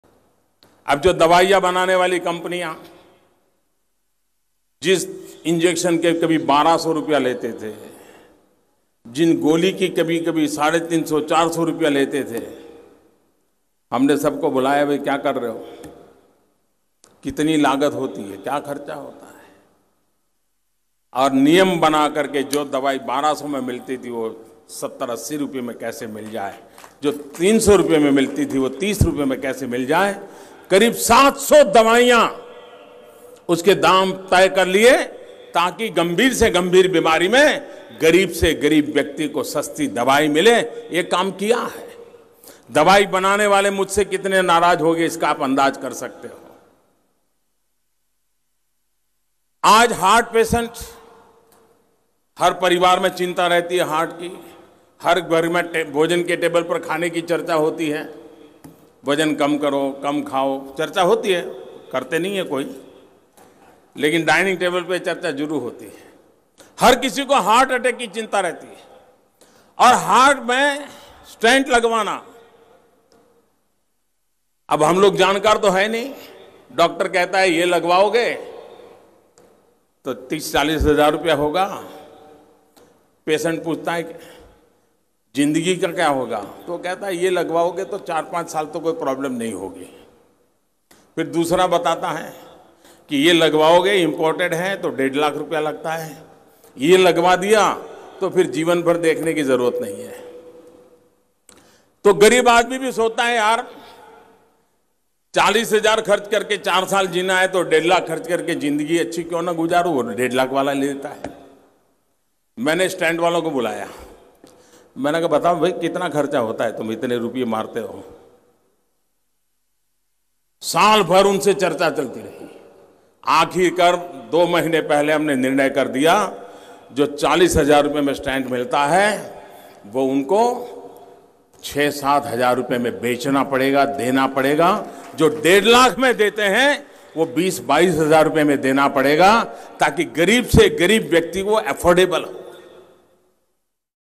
We capped prices of about 700 medicines reducing their prices to as low as 10% of existing prices
We capped the prices of about 700 medicines, reducing their prices to as low as 10% of existing prices in some cases : PM Shri Narendra Modi on healthcare reforms benefiting the poor.